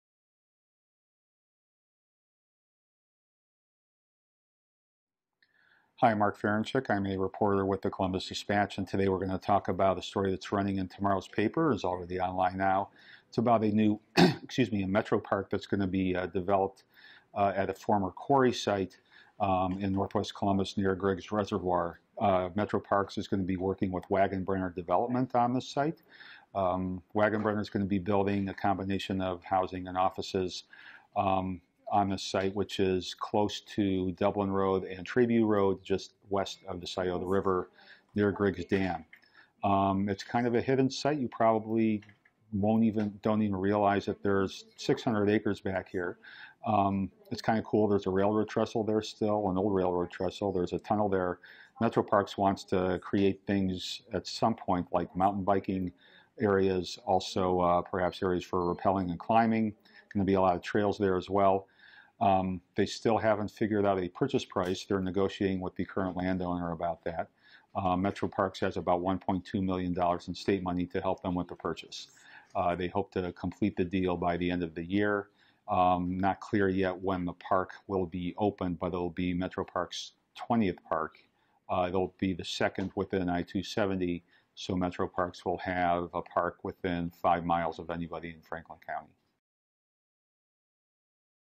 Why You Should Care: Mark Ferenchik talks possible new Metro Parks development
Originally Published 06/19/2017 - Why You Should Care: Mark Ferenchik talks possible new Metro Parks development